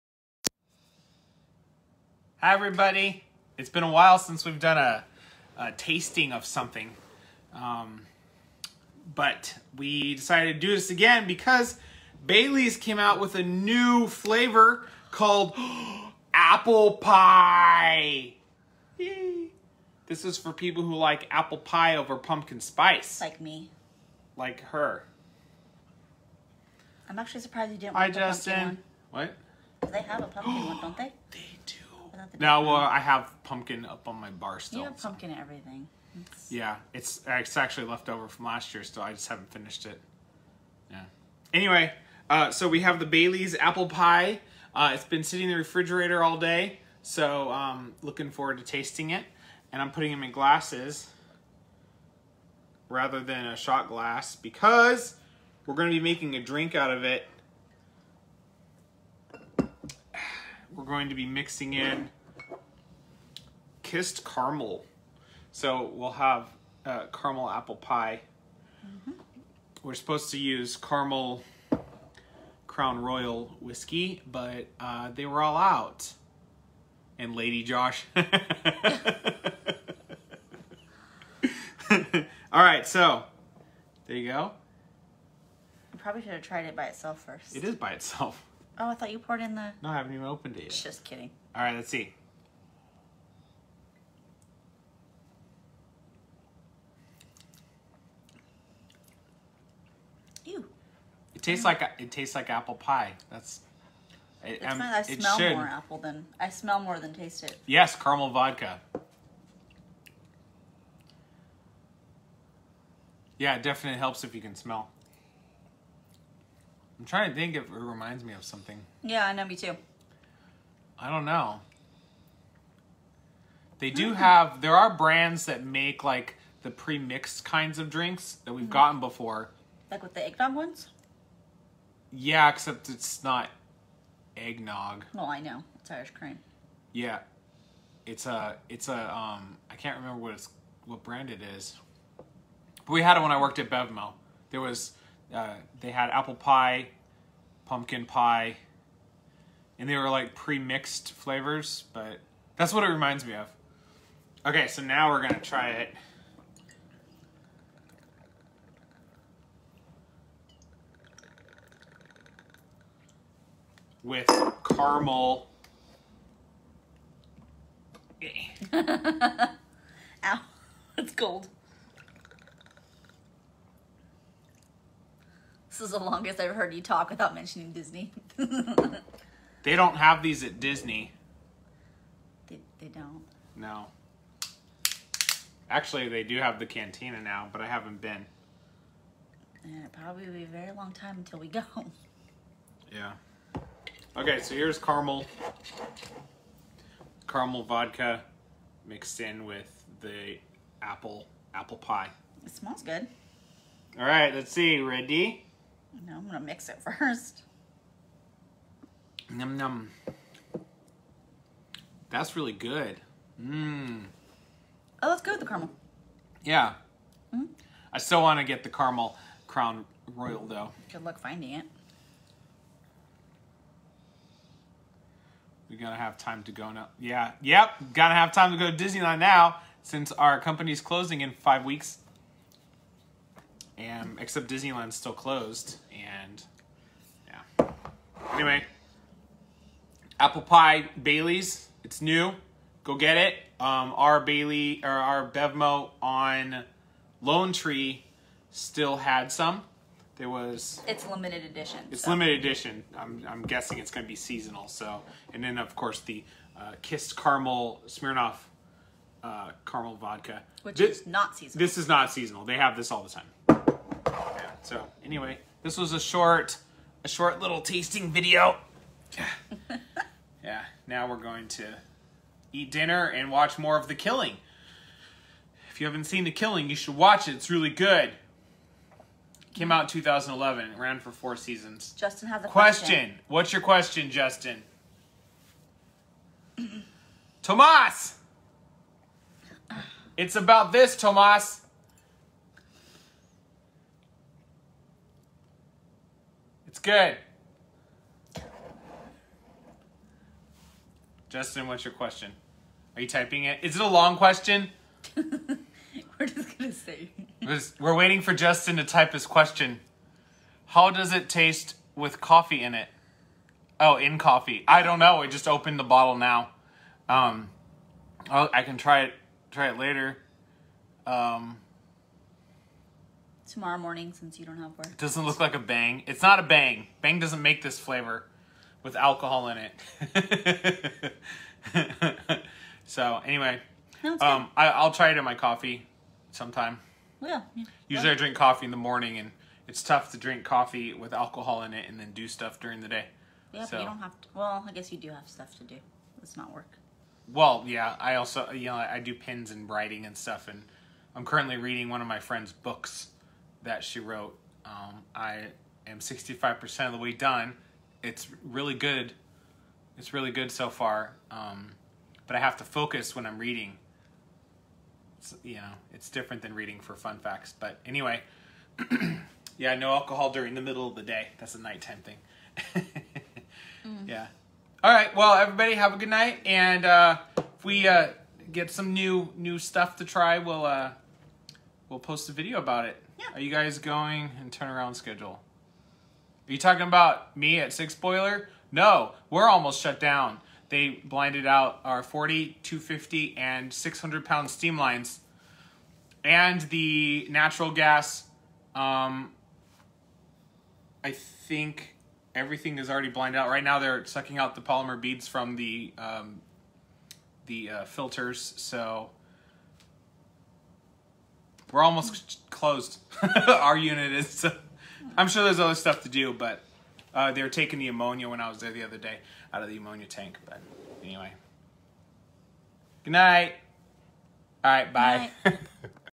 My wife and I taste-test the New Baileys' Apple Pie
Tried out the new Limited Edition Baileys' Apple Pie.
This was filmed on September 20, 2020.

We also tried it with Smirnoff Kissed Caramel Vodka mixed in.